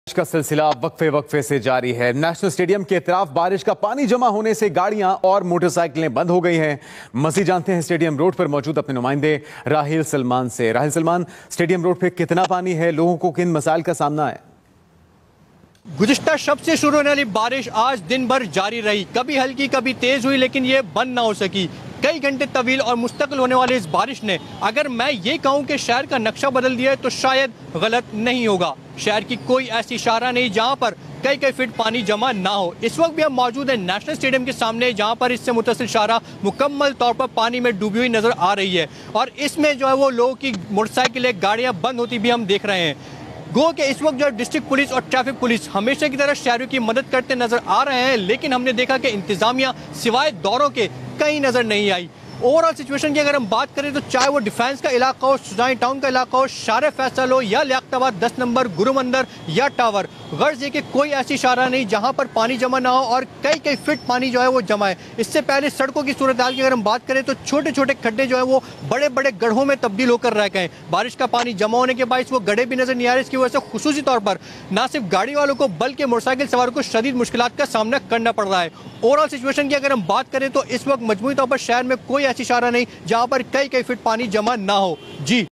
Karachi: Rain continues, vehicles and motorcycles are stopped due to water accumulation around
Karachi: Rain continues, vehicles and motorcycles are stopped due to water accumulation around the stadium